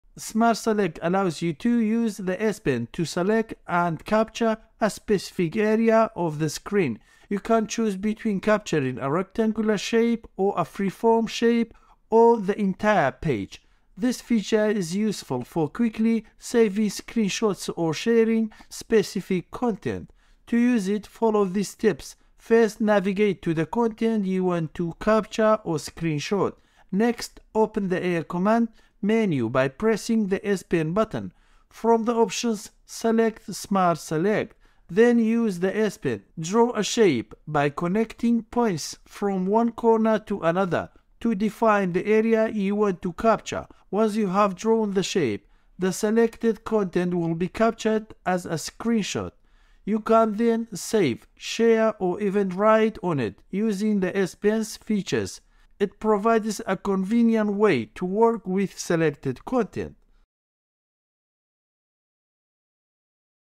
Samsung S pen Smart Select Feature
Smart Select allows you to use the S_Pen to select and capture a specific area of the screen. You can choose between capturing a rectangular shape, a free-form shape, or the entire page. This feature is useful for quickly saving screenshots or sharing specific content. To use it, follow these steps: first, navigate to the content you want to capture or screenshot. Next, open the Air Command menu by pressing the S Pen button. From the options, select 'Smart Select.' Then, using the S Pen, draw a shape by connecting points from one corner to another to define the area you want to capture. Once you've drawn the shape, the selected content will be captured as a screenshot. You can then save, share, or even write on it using the S Pen's features. It provides a convenient way to work with selected content.